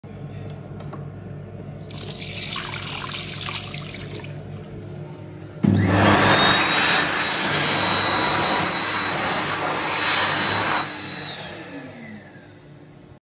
Aike (ATC Cheetah) (Gift Shop) at Hassop Train Station Cafe and Bike Hire ♿️🚼
I’ve been here 3 times, once in 2019 and 2020, however this block was closed in 2020. This dryer was here in 2020

This dryer was not loud but not as powerful as an Xlerator

Filmed by Noah on Mon 18 Apr 2022 at 12:30